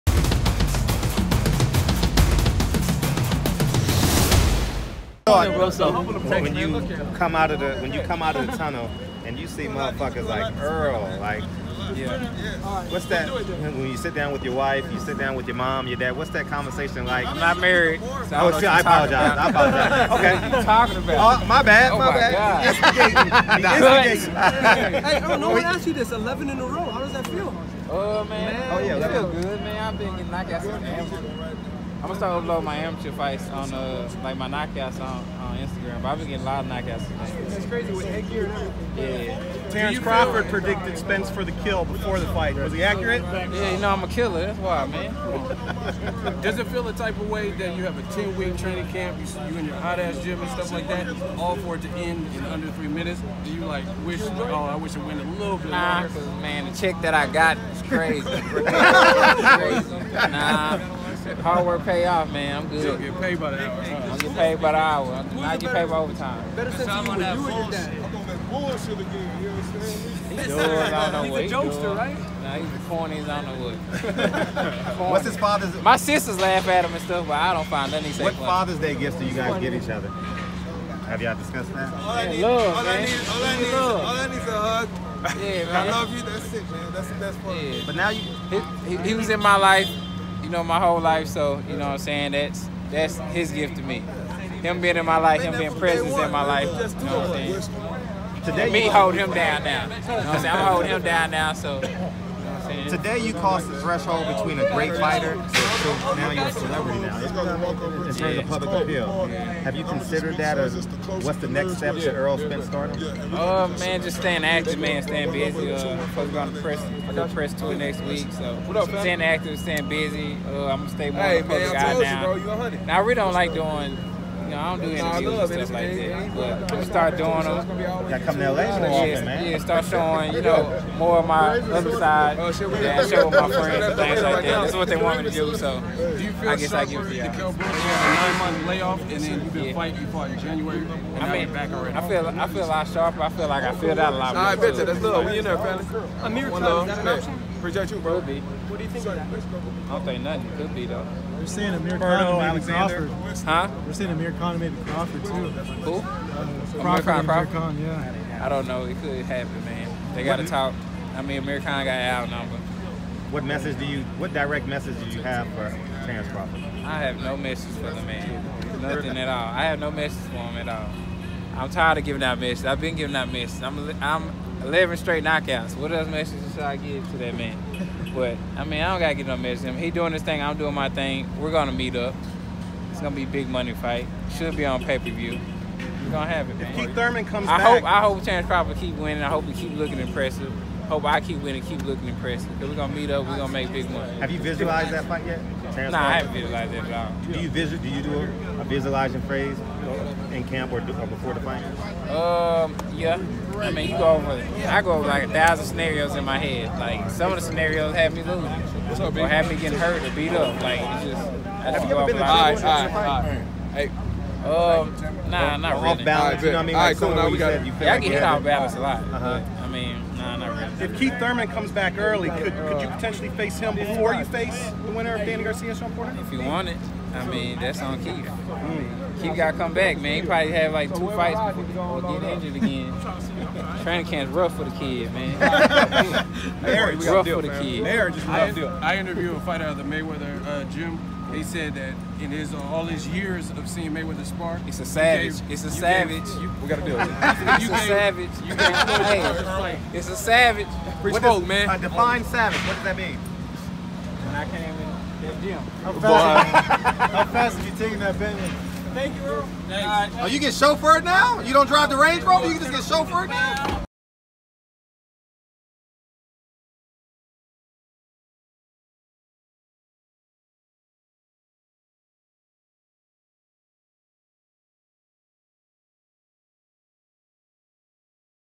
ERROL SPENCE "I HOPE TERENCE CRAWFORD KEEPS WINNING! WE ARE GONNA MEET UP & MAKE BIG MONEY!"
errol spence jr speaks on his big knockout win over carlos ocampo and a potential fight with terence crawford as well as training camp being rough on keith thurman